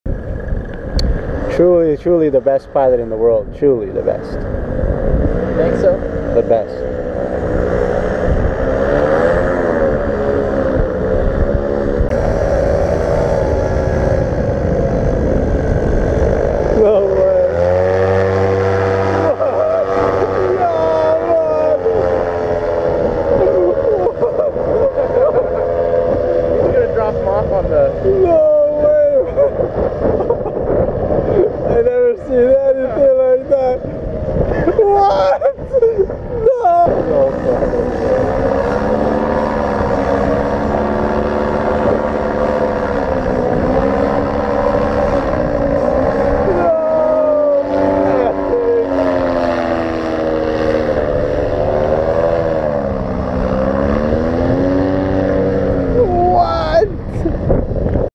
Most Dangerous Paramotor Stunt EVER!!!! You Can't Believe This!!!
Paramotor Insanity!! Do NOT try this at home boys and girls.  The best pilot in the history of paramotoring picks a guy up and drops him perfectly on top of a gazebo.  Then picks him back up and sets him back on the ground.  This is one of those things you have never seen and will probably never again see in your lifetime.